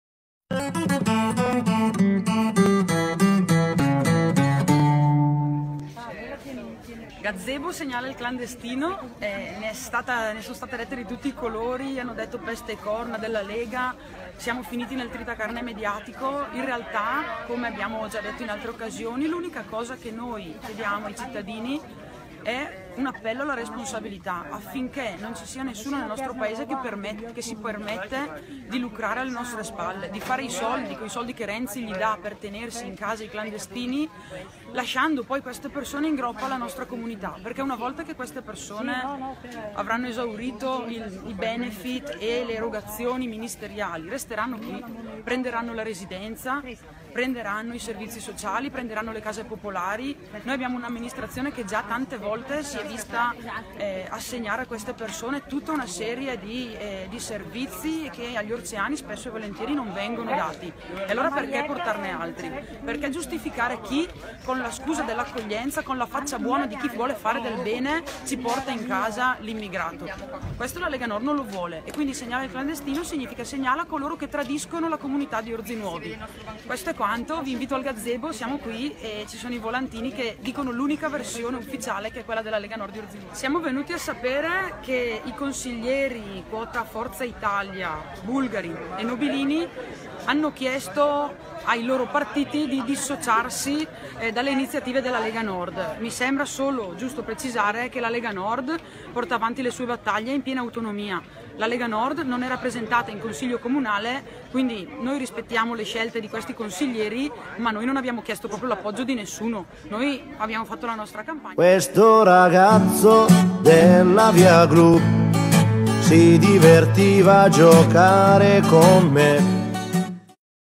ORZIDAY! - CAMPAGNA SEGNALA IL CLANDESTINO, LA VERSIONE ORIGINALE
“CAMPAGNA SEGNALAZIONE CLANDESTINI”. LA BATTAGLIA CAMPALE DELLA LEGA NORD DI ORZINUOVI PER SCONGIURARE L’ARRIVO DEI PROFUGHI IN PAESE

Orzinuovi, 28 luglio 2015 - «L’amministrazione comunale di Orzinuovi, targata PD-Renzi, dichiara di voler ospitare i presunti profughi o richiedenti asilo, ma in questo modo sta abbandonando la nostra gente proprio nel momento di maggior bisogno». Così Federica Epis, Segretario della Lega Nord di Orzinuovi, annuncia il gazebo “Campagna segnalazione clandestini” in programma per venerdì mattina 31 luglio a Orzinuovi, durante il mercato settimanale.

«La cosiddetta “accoglienza” è in realtà un business finanziato a spese di chi paga le tasse perché i nostri soldi - spiega Epis - vengono usati per pagare vitto, alloggio, sigarette e mancetta giornaliera a chi arriva qui illegalmente coi barconi. Chi ci guadagna sono enti pubblici o privati, ma anche singoli cittadini, che intascano più di mille euro al mese per ogni clandestino “accolto”. Questo è il motivo per cui lanciamo la Campagna di segnalazione».

«Orzinuovi è una comunità unita e coesa, - prosegue il Segretario del Carroccio orceano - soprattutto su questi temi non ci sono distinzioni di partito che tengano. Ognuno di noi è responsabile per il futuro del nostro paese, ognuno può e deve vigilare affinché non venga meno il vincolo di lealtà e di fiducia che ci lega gli uni agli altri. Non è accettabile - continua Epis - che qualcuno cerchi di ospitare i clandestini solo per fare palanche scaricando i costi economici e sociali su tutti gli altri. La Lega Nord - conclude - farà il possibile e l’impossibile per impedire l’invasione».